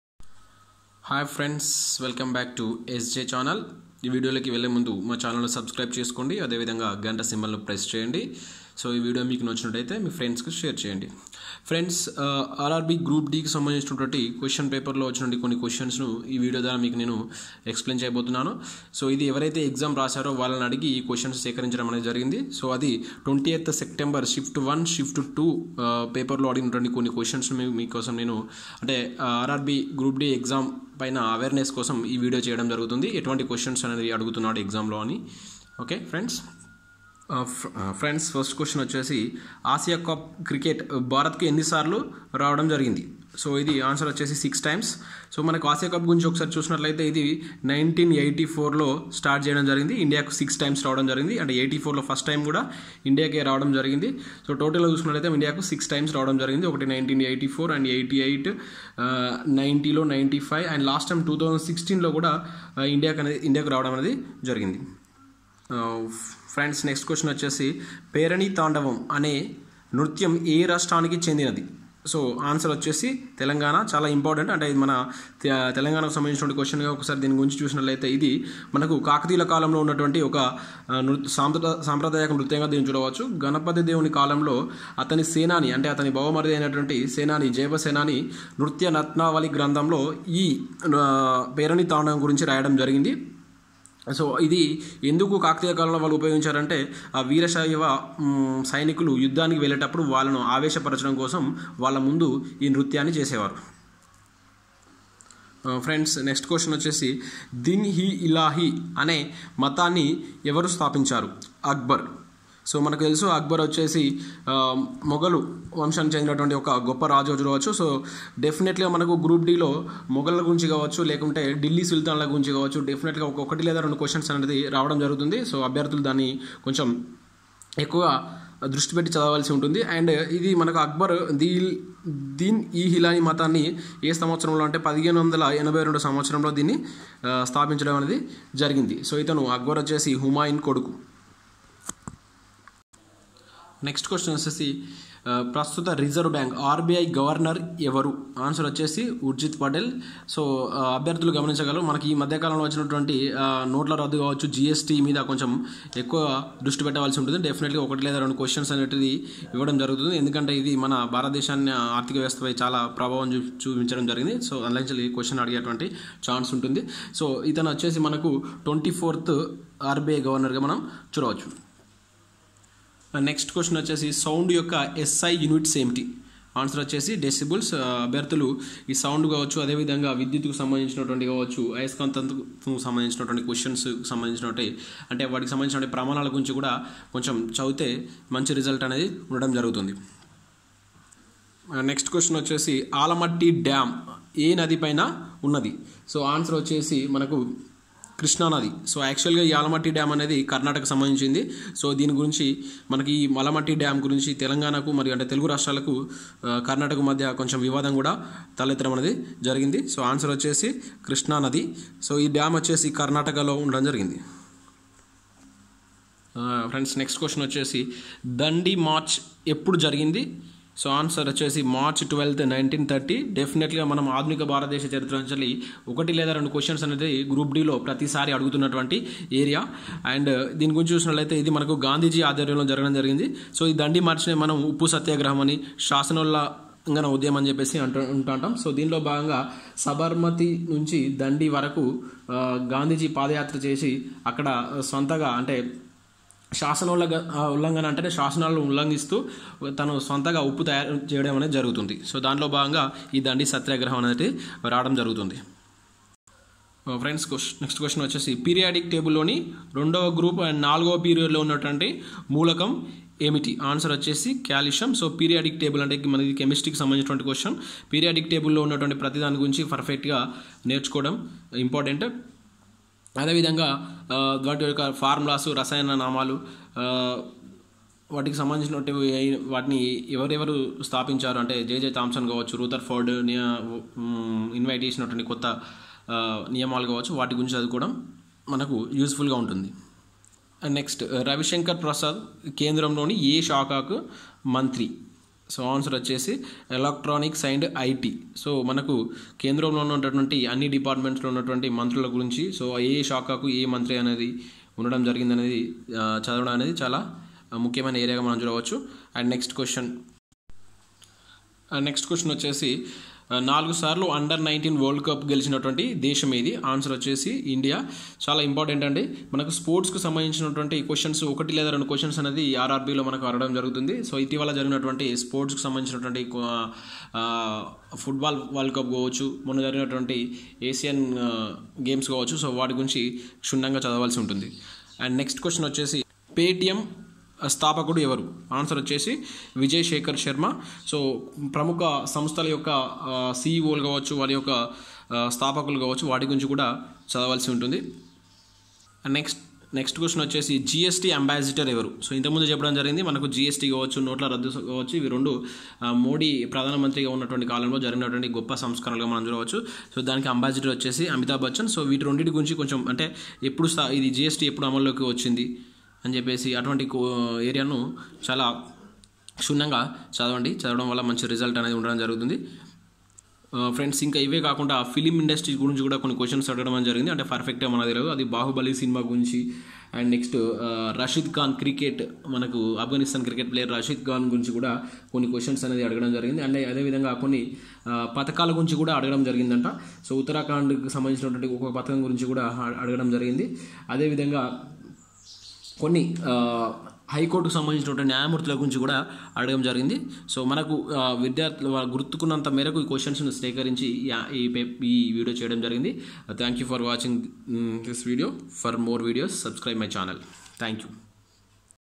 RRB GROUP D Question paper with answers in Telugu || 20th September Shift
RRB Shift 1 & Shift 2 లో అడిగిన ప్రశ్నలు